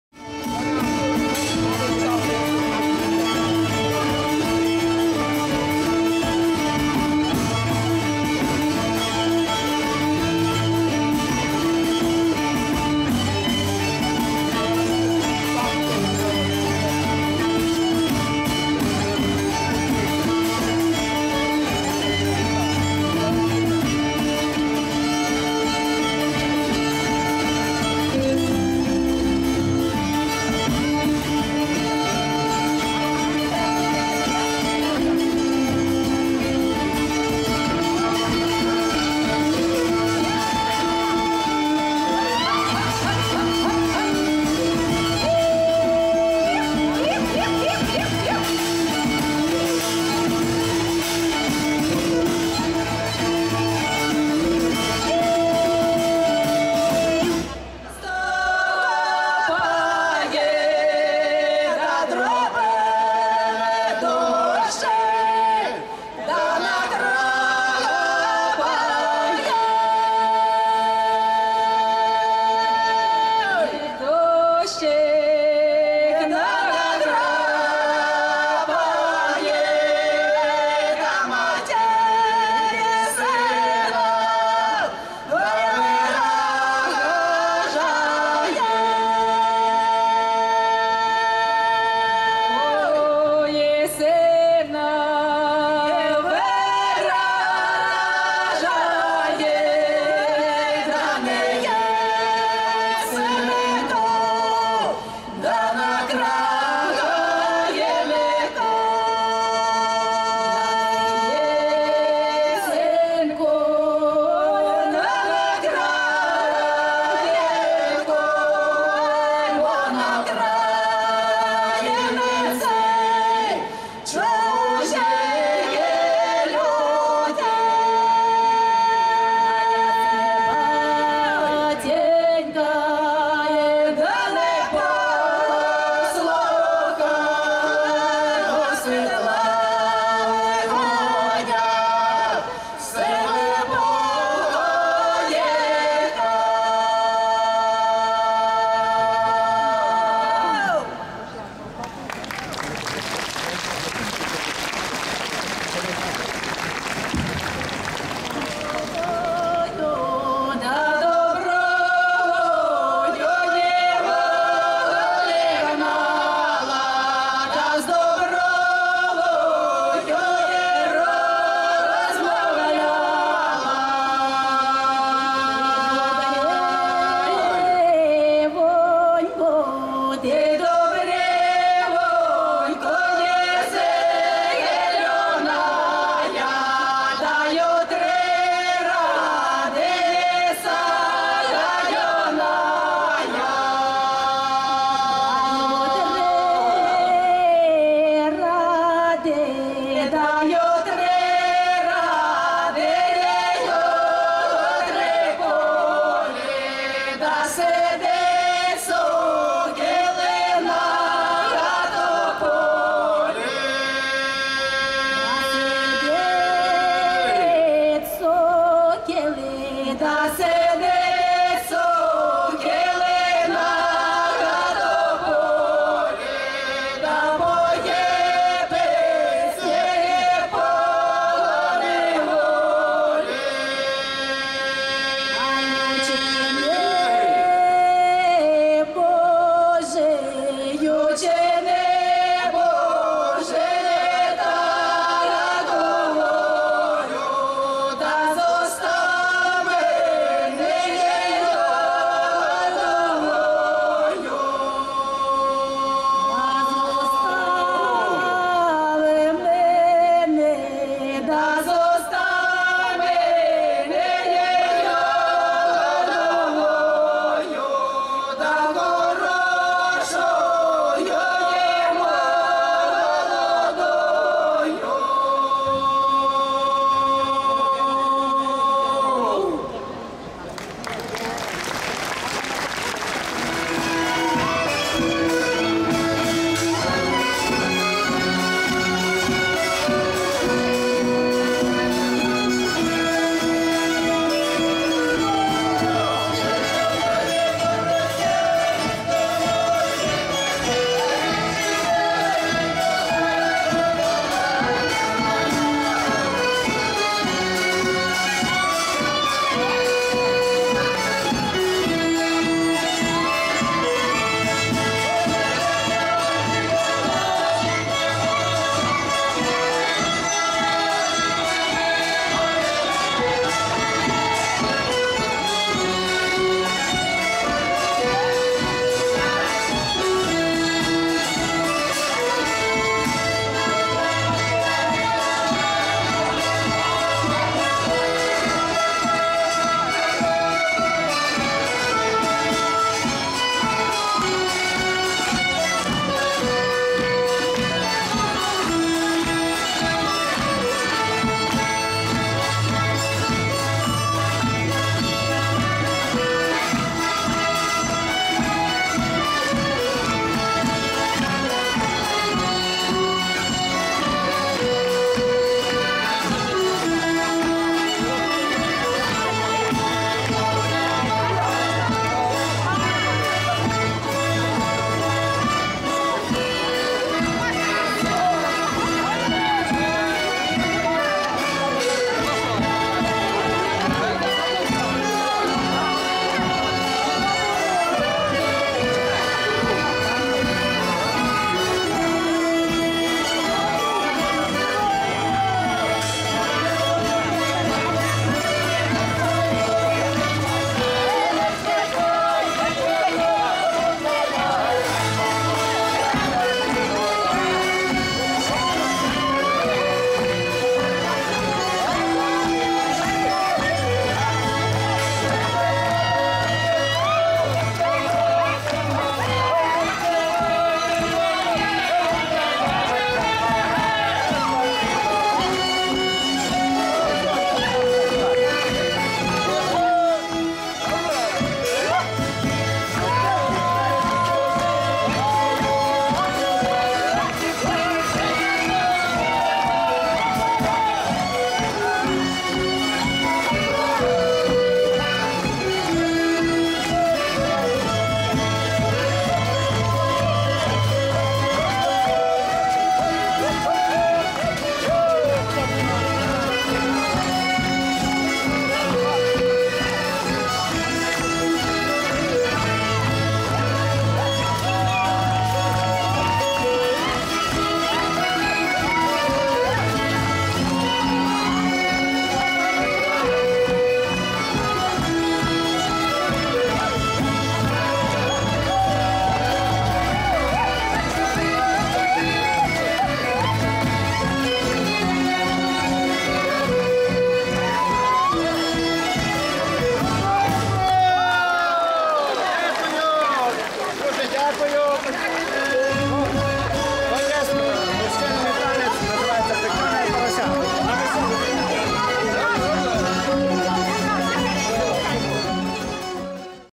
SILSKA MUZYKA - Ukraina - XI Podlaska Oktawa Kultur - Białystok 26.07.2018
Białystok - XI Międzynarodowy Festiwal Muzyki, Sztuki i Folkloru „Podlaska Oktawa Kultur” - Silska Muzyka (Ukraina) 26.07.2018